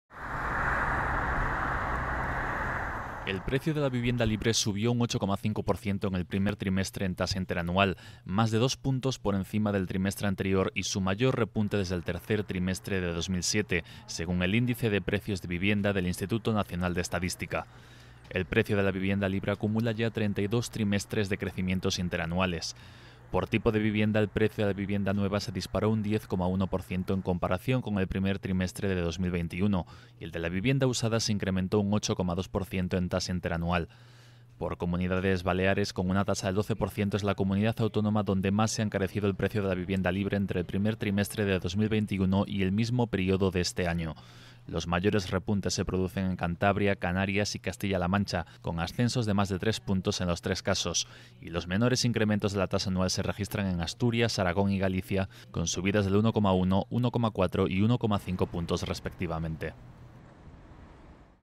El precio de la vivienda libre se dispara un 8,5% en el primer trimestre
El precio de la vivienda libre subió un 8,5% en el primer trimestre en tasa interanual, más de dos puntos por encima del trimestre anterior y su mayor repunte desde el tercer trimestre de 2007, según el Índice de Precios de Vivienda (IPV) del Instituto Nacional de Estadística (INE).